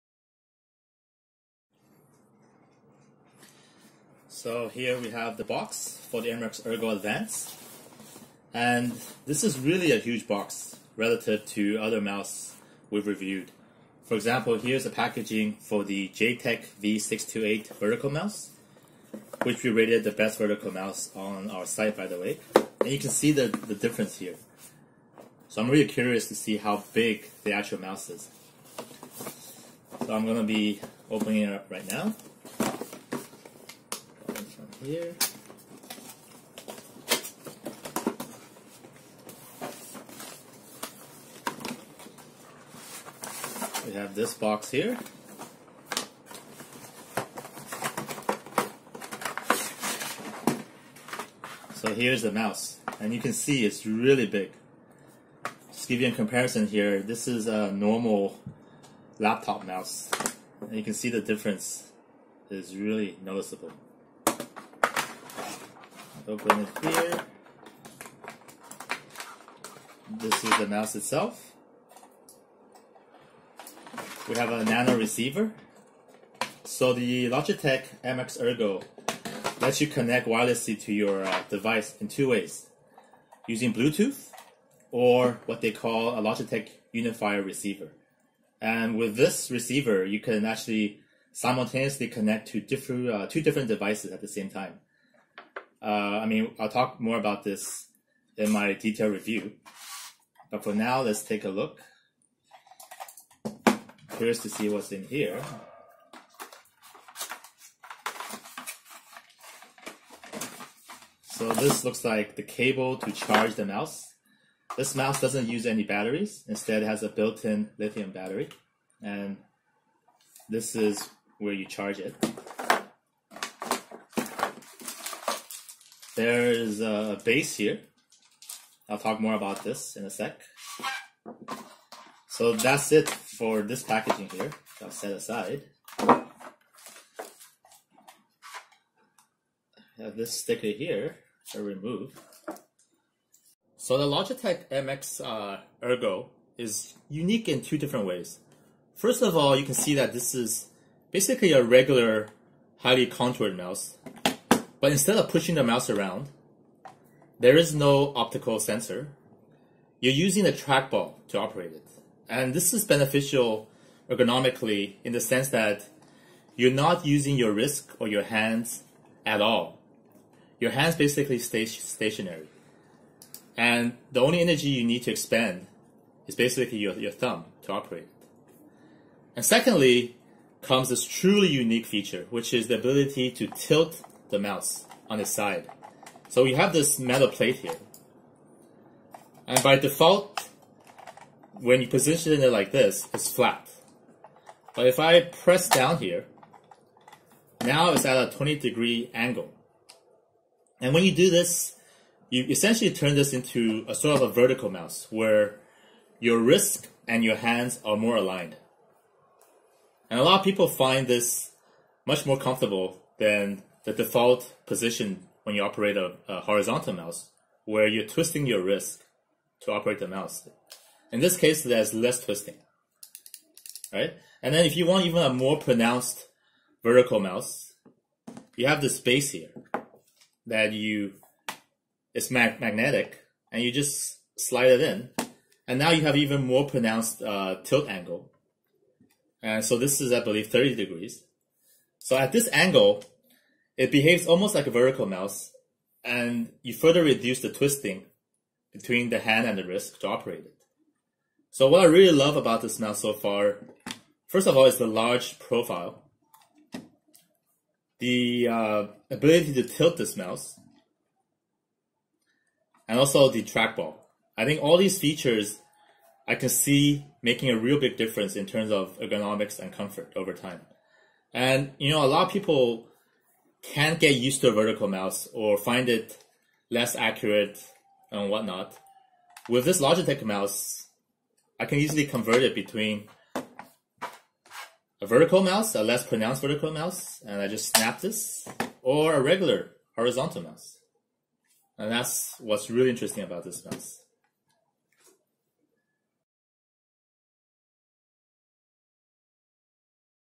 Logitech MX Ergo Plus unboxing
Logitech MX Ergo Plus is our pick for the best ergonomic mouse for 2018. See my unboxing of this mouse, plus detailed review of it:

What makes the Logitech MX ERGO such an ergonomic mouse? It’s all in the adjustable hinge that lets you tilt the mouse up to 20 degrees on its side for the most natural, comfortable position. This slight change in angle may not seem like much, but overtime it makes a huge difference for a lot of people. By minimizing any unnatural twists to your wrist while you use the mouse, you greatly reduce your chances of developing common ailments such as nerve pain, stiffness in the fingers and arms, and even carpal tunnel syndrome. No other horizontal mouse we’ve reviewed so far has this feature.